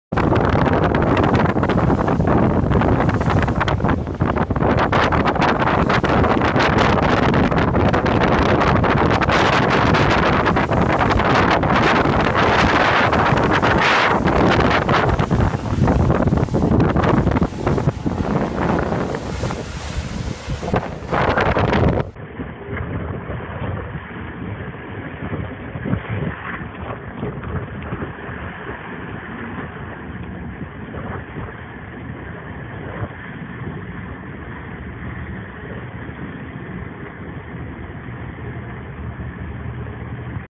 Search continues for 3 from report of downed aircraft off Molokai
(U.S. Coast Guard video/Released)